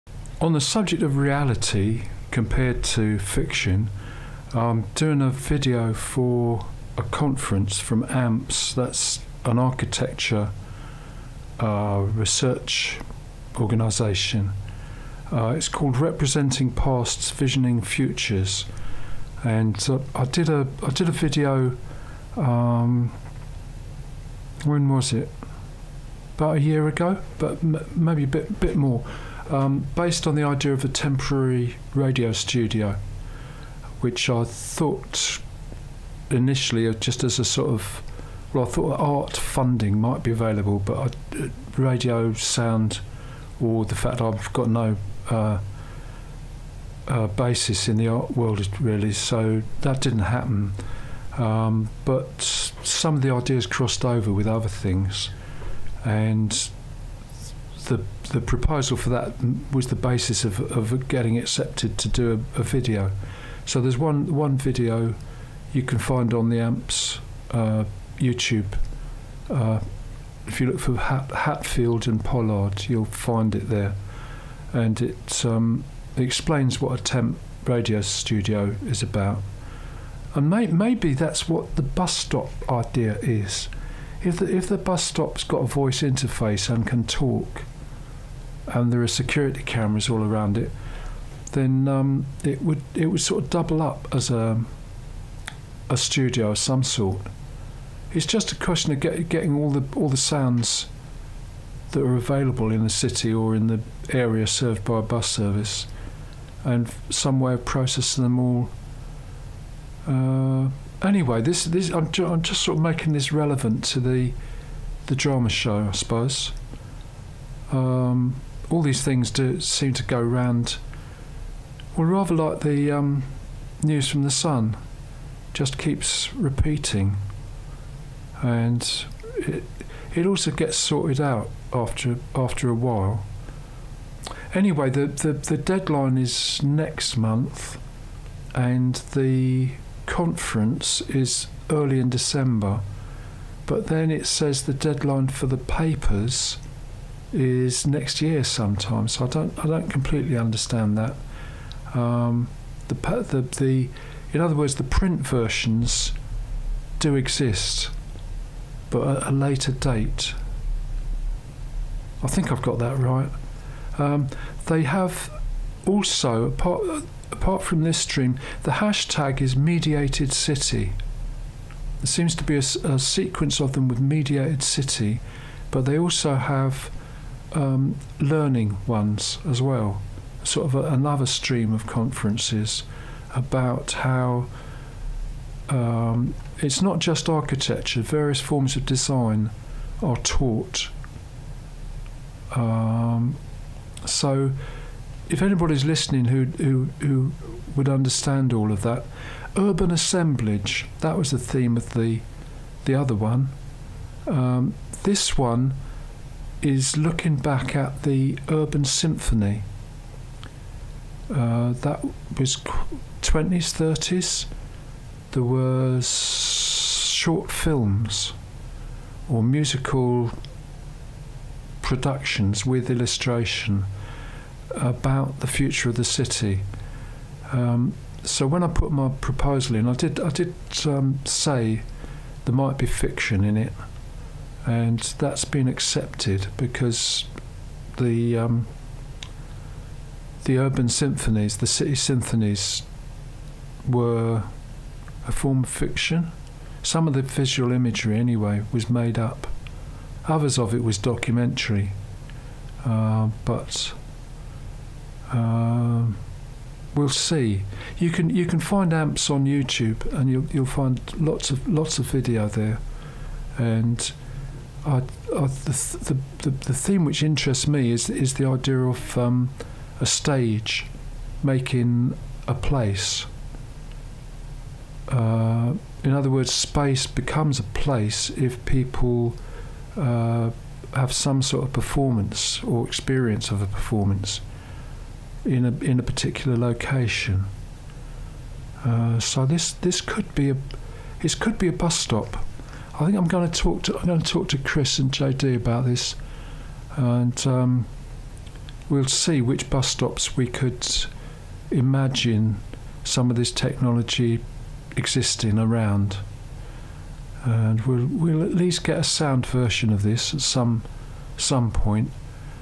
AMPS video comment - Clip from Drama show
@will789gb explains the video for conference on past and future of the city.